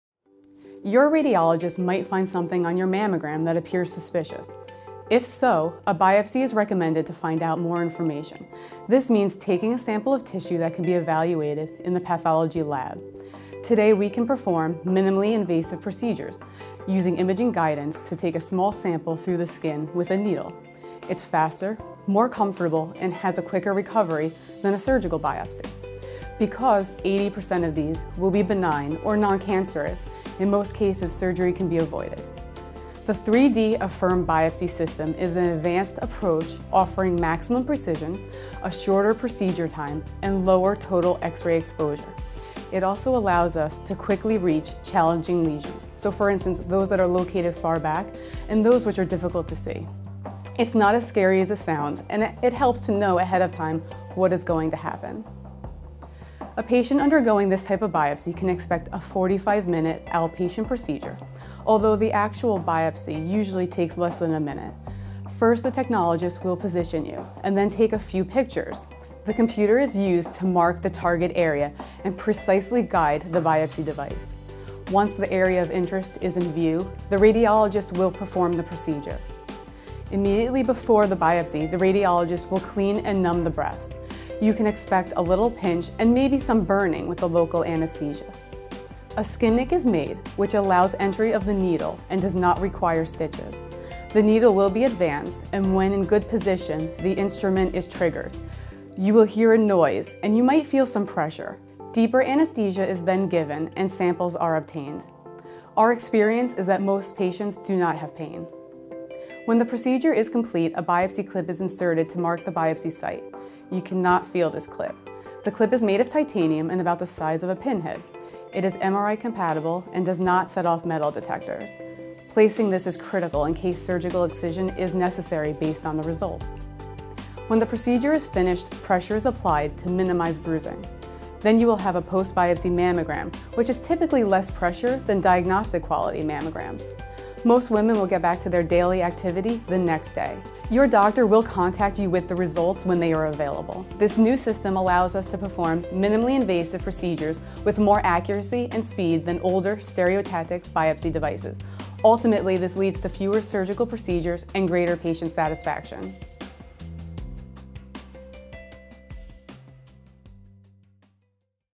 Carolyn Malone, M.D., discusses benefits of 3D Affirm Biopsy
The 3D Affirm Biopsy system is an advanced approach offering maximum precision, a shorter procedure time and lower total x-ray exposure. According to Carolyn Malone, M.D., a board certified diagnostic radiologist, the 3D Affirm Biopsy also allows the care team at Hackensack University Medical Center to quickly reach challenging lesions, for instance those located far back and those which are difficult to see.